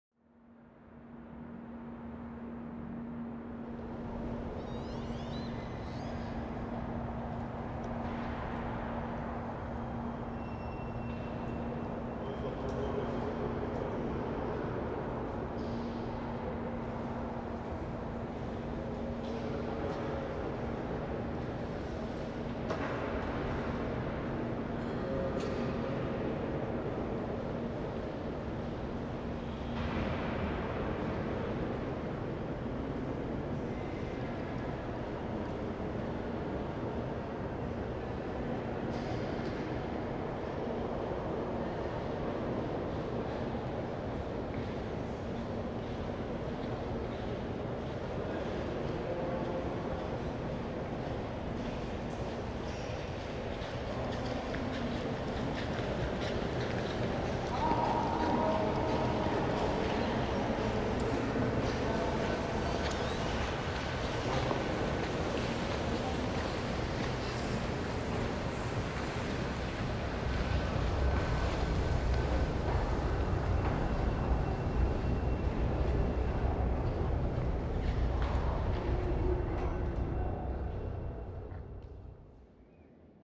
Art Web Radio : Claude Lévêque, Hymne, 2006
Claude Lévêque, Hymne, 2006
Installation in situ, Hamburger Bahnhof, Museum für Gegenwart, Berlin.
Lames inox polymiroir pivotantes, altuglas noir, lampes à iodure métallique, ventilateurs
Photos Thomas Bruns
Réalisation Armand Morin. Production Kamel mennour et Lab-Labanque